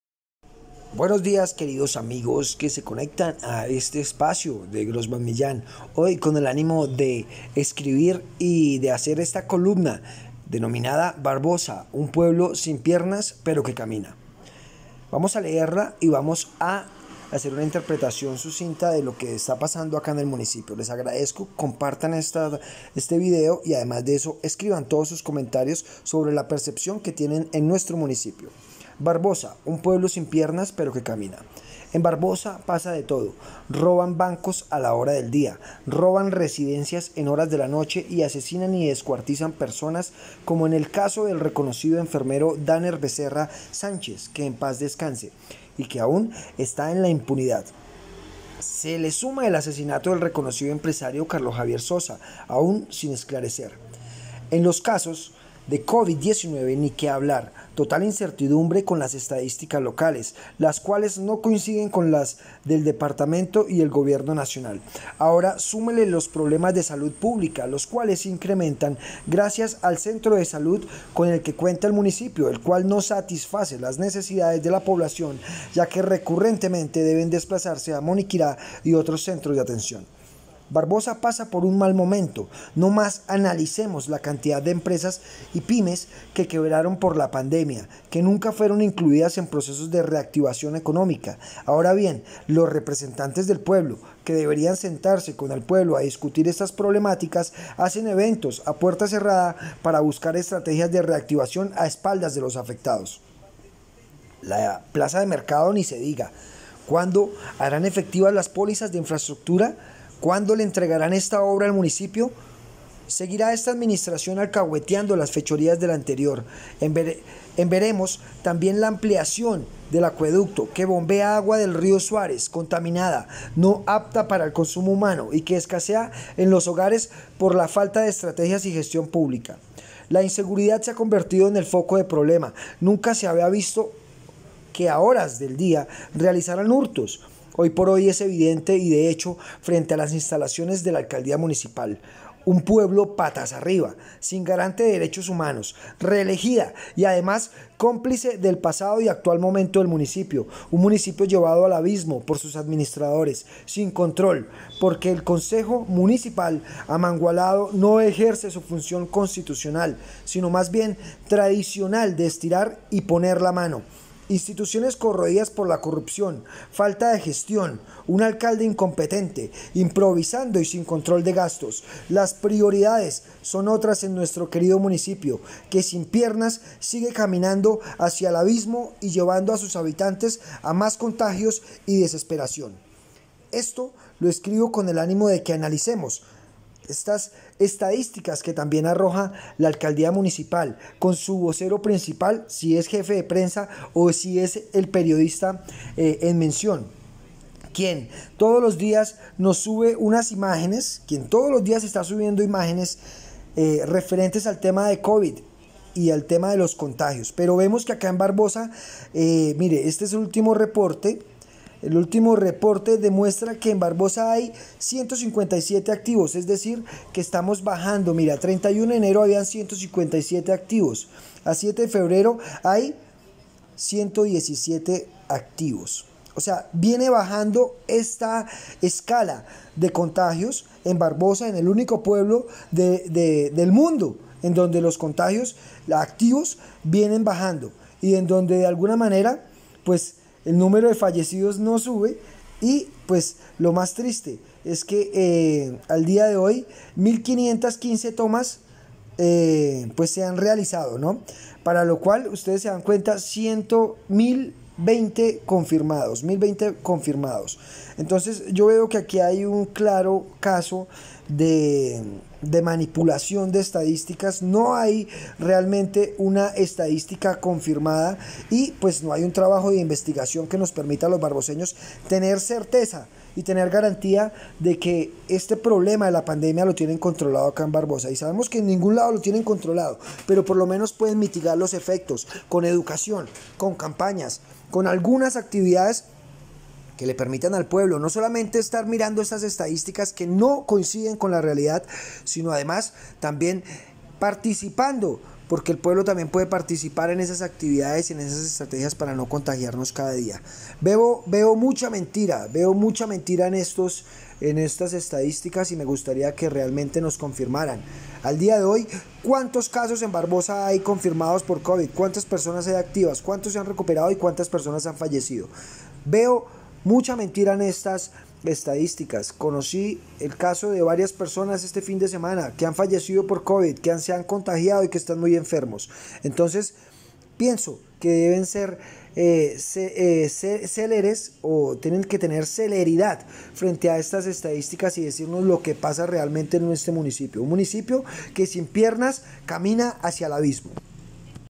Barbosa Santander, Un Pueblo sin Piernas pero que Camina.
🇭🇺 En Barbosa pasa de todo. Roban bancos a la hora del día, roban residencias en horas de la noche, asesinan y descuartizan personas como el caso del reconocido enfermero Danner Becerra Sánchez (Q.E.P.D) y que aún está en la impunidad. Se le suma el asesinato del reconocido empresario Carlos Javier Sosa aún sin esclarecer.

En los casos de Covid-19 ni que hablar: total incertidumbre con las estadísticas locales, las cuales no coinciden con las del Departamento y Gobierno Nacional. Ahora súmele los problemas de salud pública, los cuales incrementan gracias al Centro de Salud con el que cuenta el municipio, el cual no satisface las necesidades de la población, ya que recurrentemente deben desplazarse a Moniquirá y otros centros de atención.

Barbosa pasa por un mal momento, no más analicemos la cantidad de empresas y pymes que quebraron por la pandemia, que nunca fueron incluidas en procesos de reactivación económica. Ahora bien, los representantes del pueblo, que deberían sentarse con el pueblo a discutir estás problemáticas, hacen eventos a puerta cerrada para buscar estrategias de reactivación a espaldas de los afectados.

La plaza de mercado ni se diga. ¿Cuándo harán efectivas las pólizas de infraestructura? ¿Cuándo le entregarán está obra al municipio? Seguirá está administración alcahuetiando las fechorías de la anterior. En veremos también la ampliación del acueducto, que bombea agua del río Suárez, contaminada no apta para el consumo humano y que escasea en los hogares por la falta de estrategias y gestión pública.

La inseguridad se ha convertido en el foco problema, nunca se había visto que a horas del día realizarán hurtos. Hoy por hoy es evidente y de hecho, frente a las instalaciones de la alcaldía municipal. ¡Un pueblo patas arriba! Sin garante de DD.HH, reelegida y además, cómplice del pasado y actual momento del municipio. Un municipio llevado al abismo por sus administradores, sin control, porque el concejo municipal, amangualado no ejerce su función constitucional, sino más bien tradicional de estirar y poner la mano.

Instituciones corroidas por la corrupción, falta de gestión, un alcalde incompetente improvisando y sin control de gastos. Las prioridades son otras en nuestro querido municipio, que sin piernas sigue caminando hacia el abismo y llevando a sus habitantes a más contagios y desesperación.